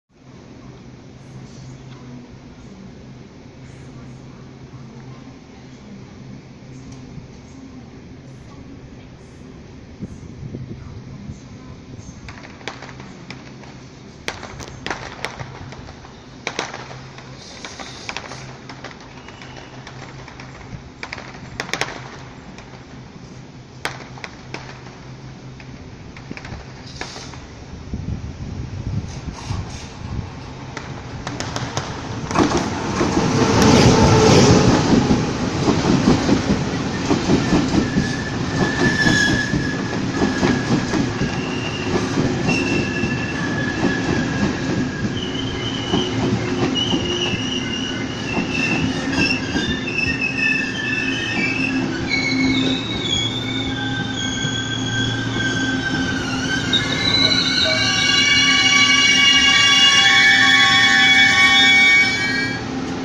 123次經由山線開往屏東的PP自強號
109/6/26 嘉義站拍攝的123次經由山線開往屏東的PP自強號